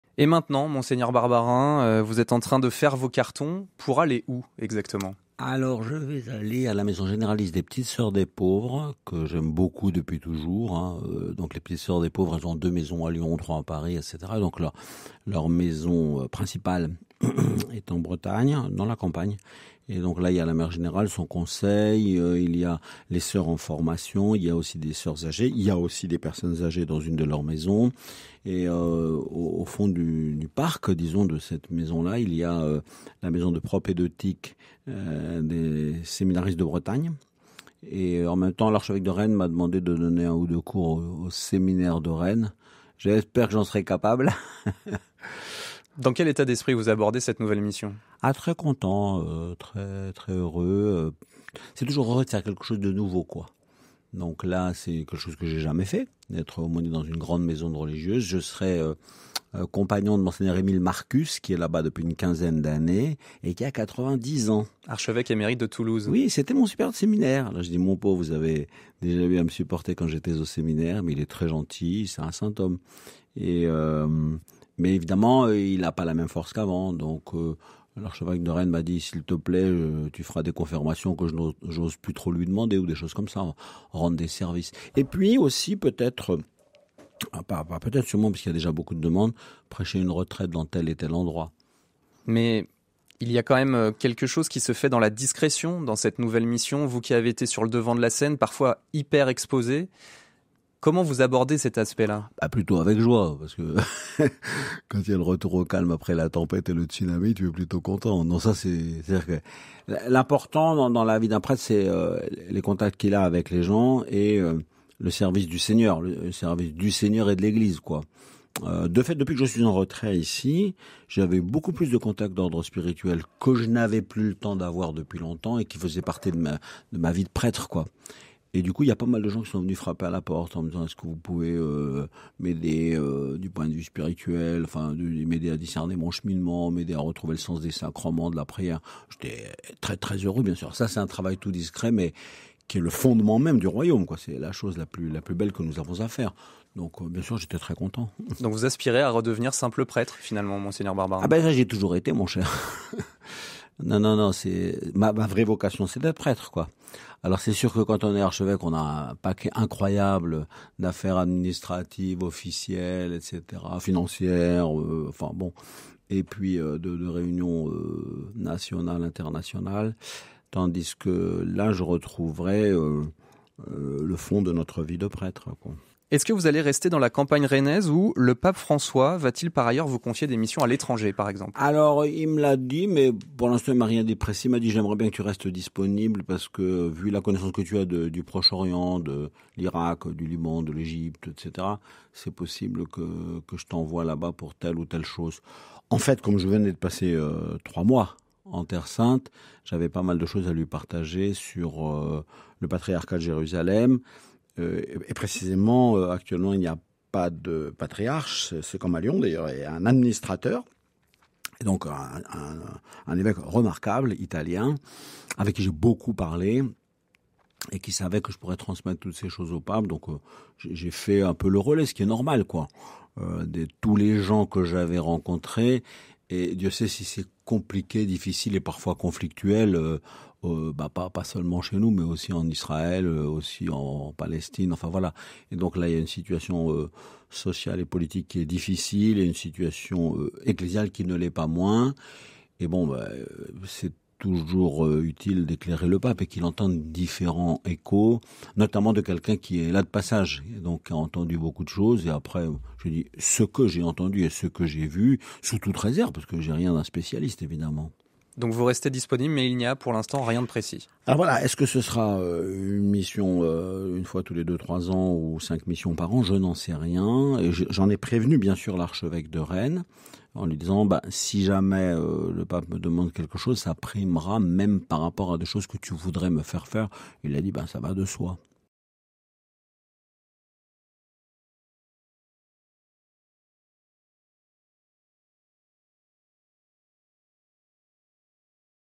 Mgr Philippe Barbarin annonce son départ pour le diocèse de Rennes - RCF Lyon
Mgr Philippe Barbarin, l'archevêque émérite de Lyon va devenir aumônier de la maison-mère des Petites Sœurs des pauvres, dans le diocèse de Rennes.

RCF, la joie se partage !